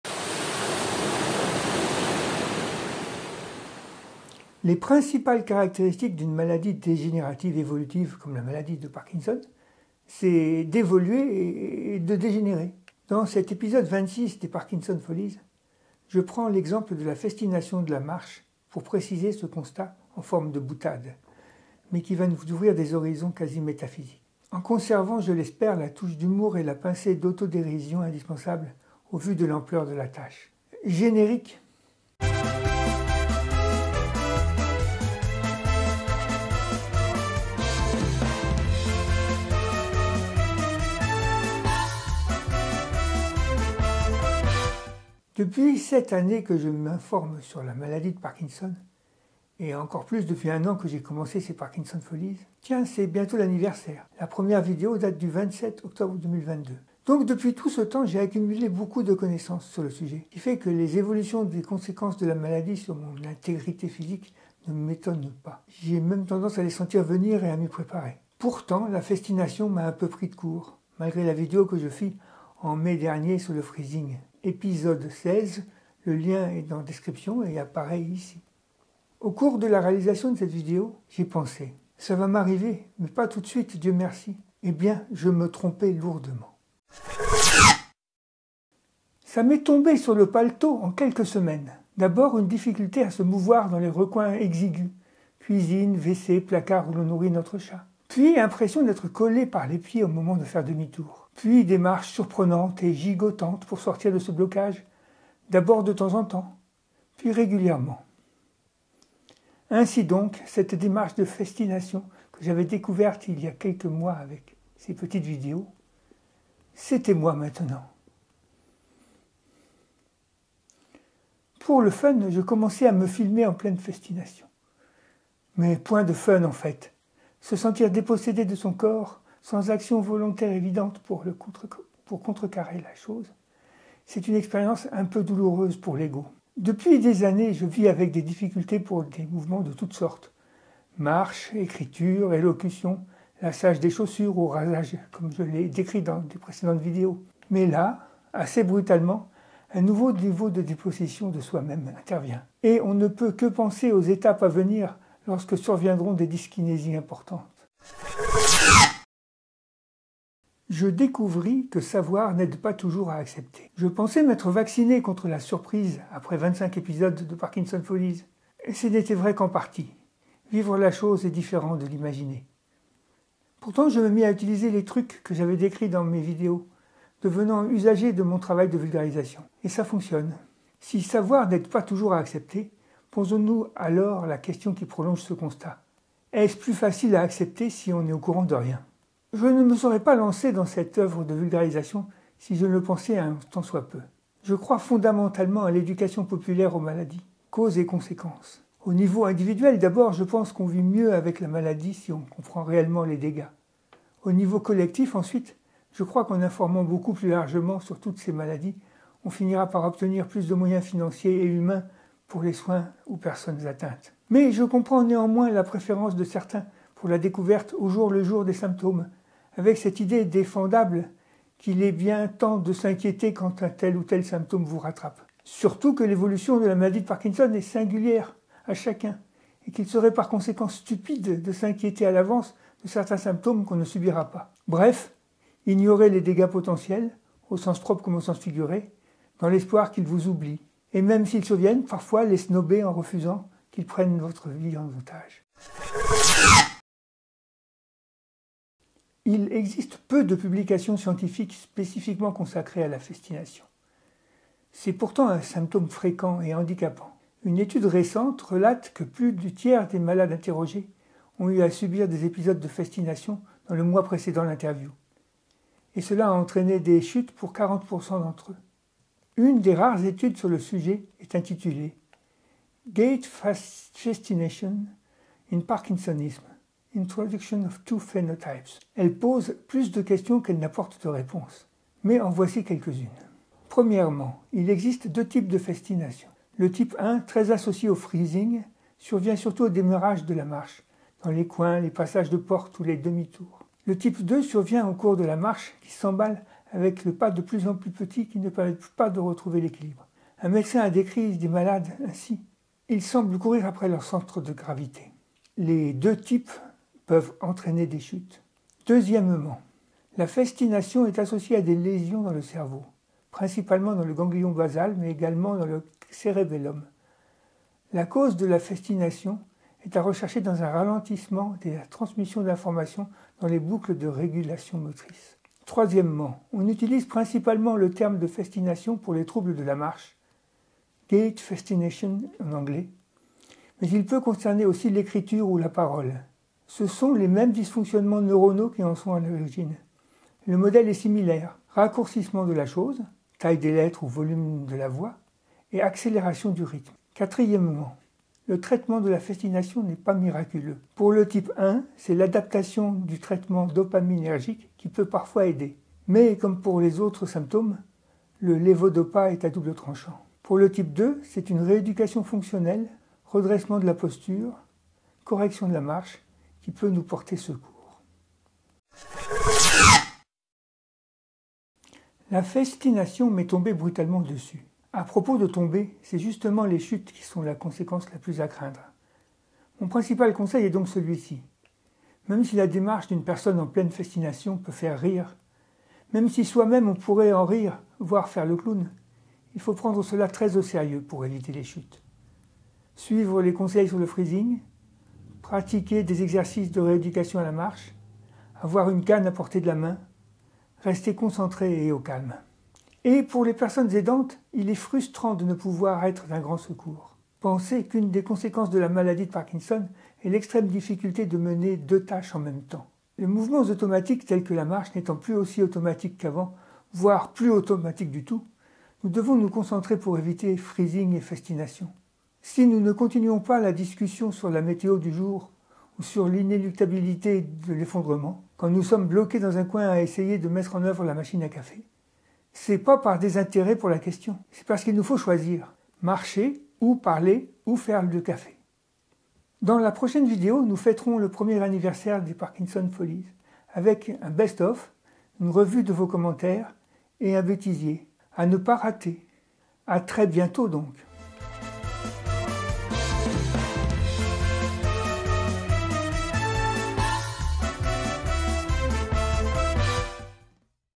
Parkinson's Follies 025 : Parkinson et festination
Les principales caractéristiques d’une maladie dégénérative évolutive comme la maladie de Pakinson, c’est …….d’évoluer et de dégénérer !
Dans cet épisode 25 des Parkinson’s Follies, je prends l’exemple de la festination de la marche pour préciser ce constat en forme de boutade mais qui va nous ouvrir des horizons quasi métaphysiques !
En conservant je l’espère la touche d’humour et la pincée d’autodérision indispensables au vu de l’ampleur de la tâche.
Gait festinating, parkinson's disease.

00:00 Introduction
01:28 1-une depossession express !
02:38 2- savoir n'aide pas toujours à accepter
04:06 3- la festination, état des connaissances
06:09 conclusion